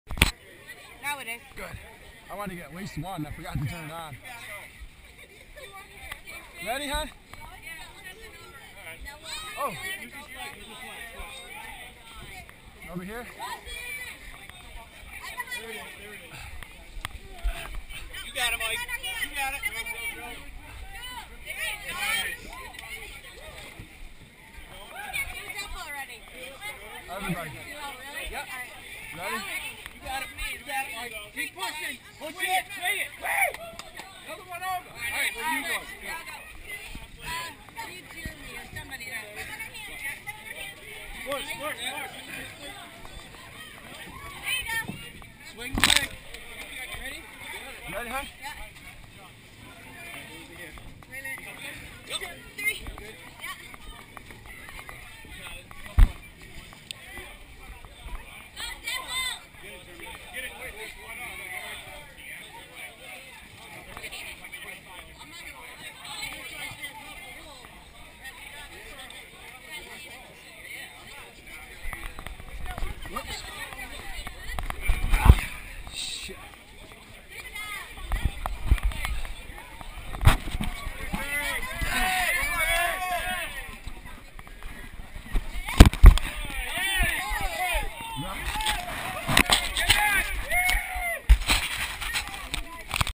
Tri-State Tough Mudder 2012 - Team Dirty Mudder Fuggers - Berlin Walls 1
The Berlin Walls 1. DMF tore this obstacle up. I would have to say we trained on walls taller on the elementary school playground. Rob made it look effortless. Hop -- over.